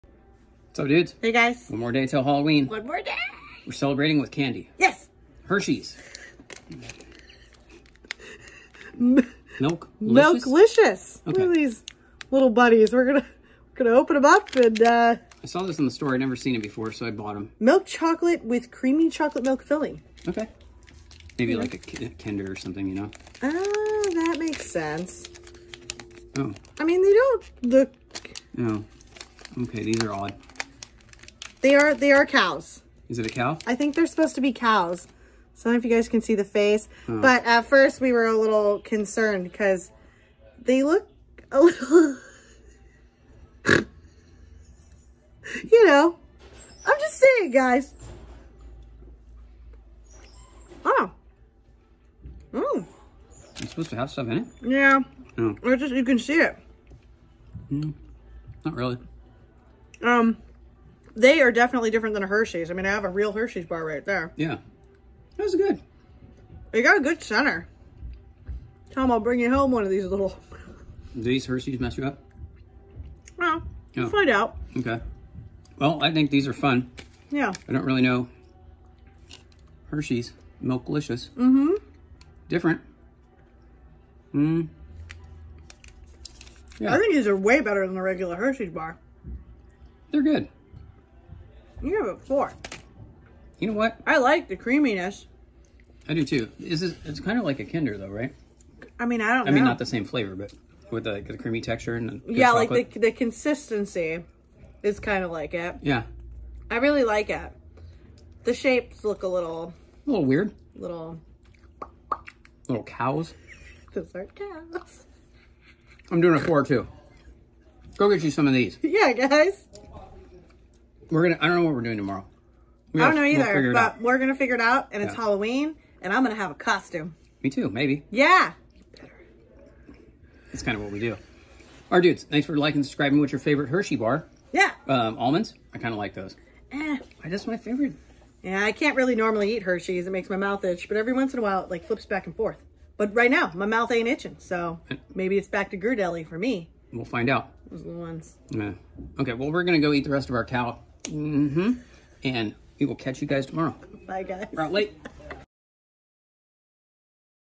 New? Hershey’s Milklicous candy bar review
Hershey’s Milklicious candy review

Not sure if these are new, but we’ve never seen them!

What’s your favorite @hersheys ?